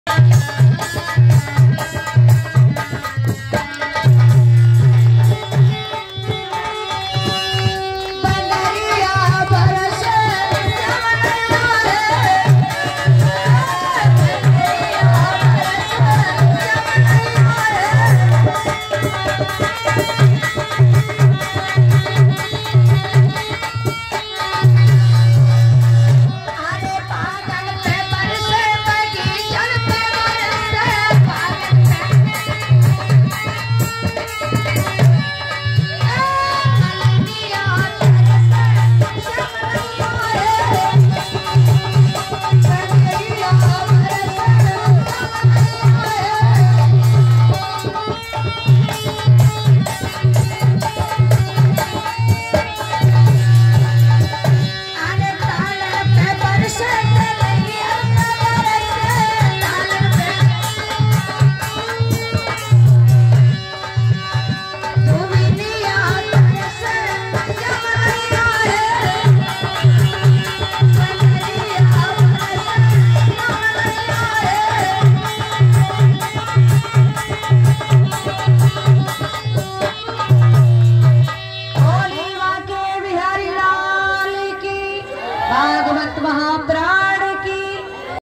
जिला बुलंदशहर में अवंतिका देवी पर भागवत कथा गायक सुपरहिट अंजली शास्त्री जी की पहुंची हजारों जनता
उत्तर प्रदेश